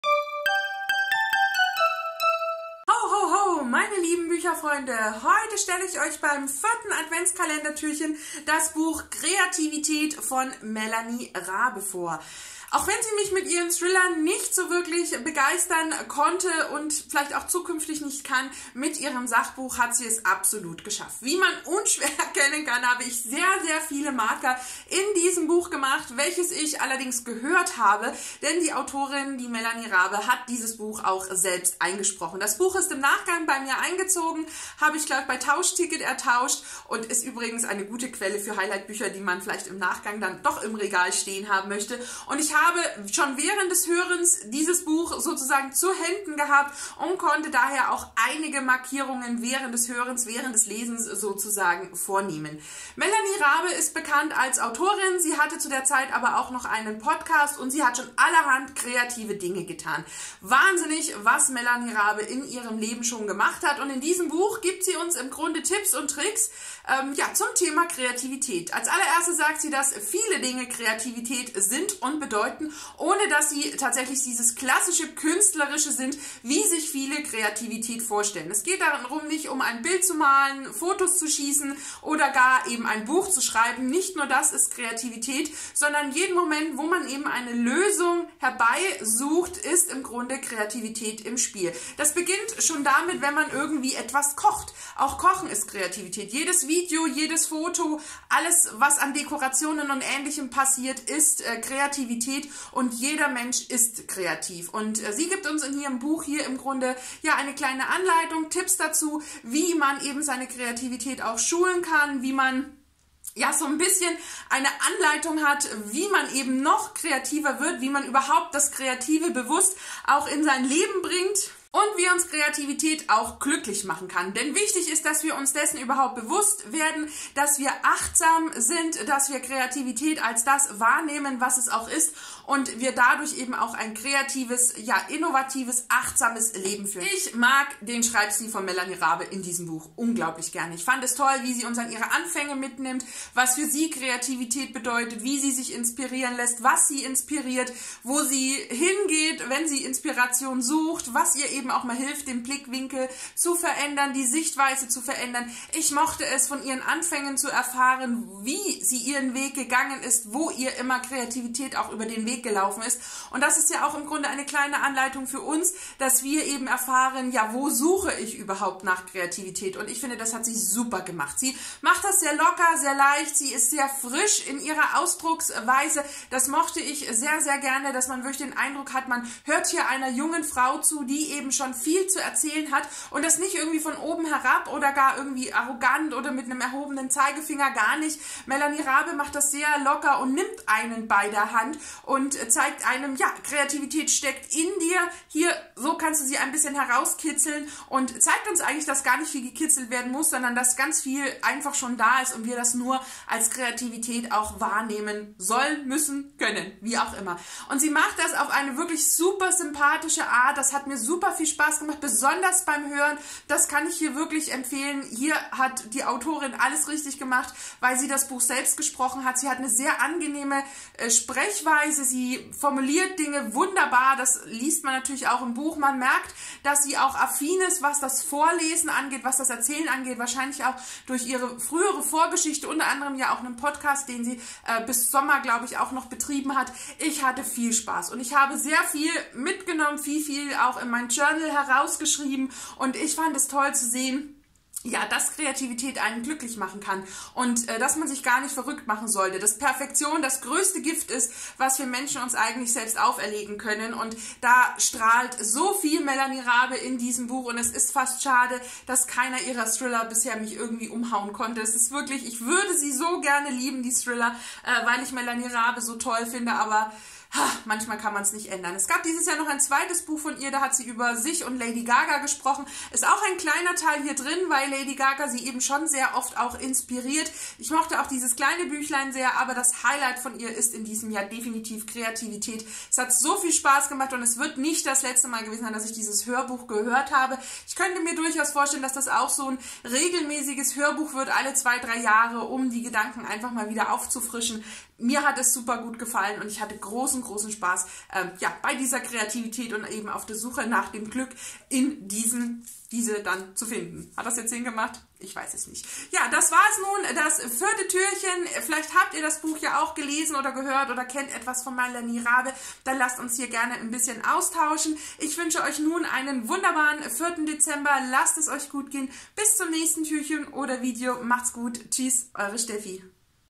Kreativität ~ JahresHighlight2021 | LadyoftheBooks
-- Öffne mich --
In diesem Video stelle ich euch ein Highligh Buch  aus 2021 vor und freu mich auf den Austausch in den Kommentaren.

„Kreativität“* von Melanie Raabe
Zum Buch**